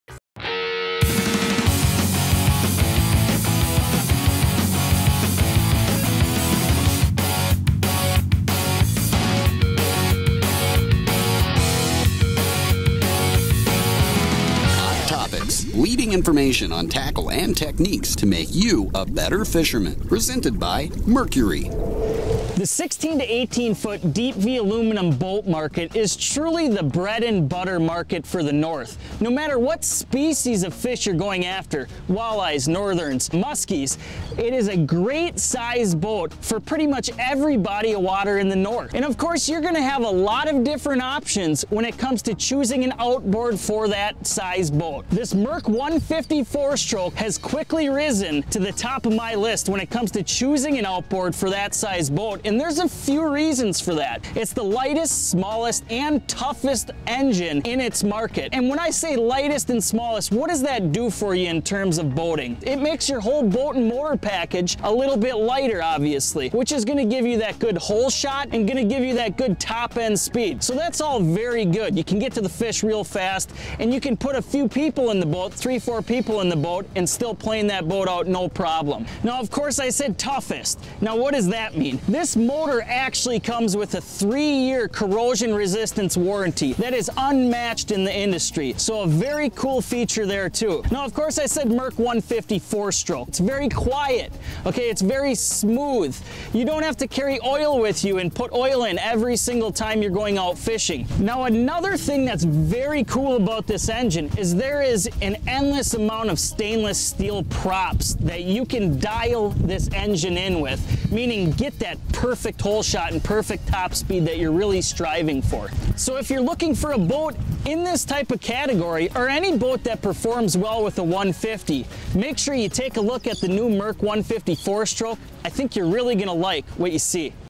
Fishing Tip - Mercury FourStroke 150HP S10E06 HT
Chase Parsons runs through the features and attributes of the Mercury 150HP FourStroke motor.